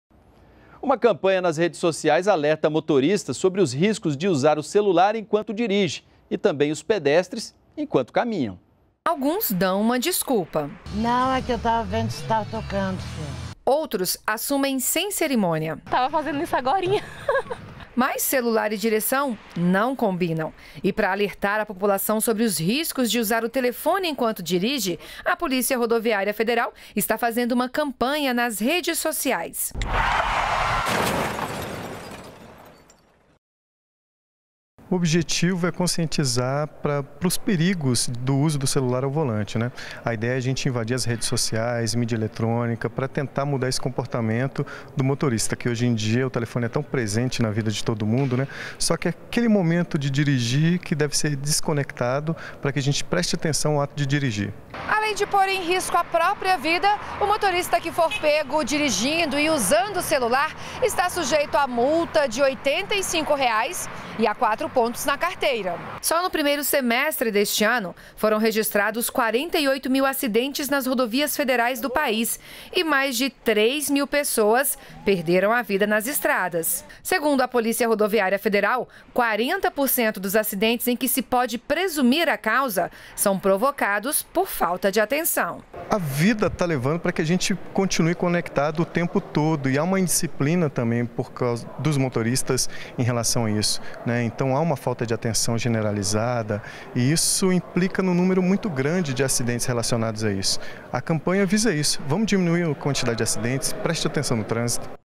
Polícia Federal promove campanha contra o uso do celular no trânsito
A Desconecta Trânsito conscientiza motoristas e pedestres sobre o risco de vida que o uso do telefone pode causar. No primeiro semestre de 2016 foram registrados 48 mil acidentes e 3 mortes em rodovias federais.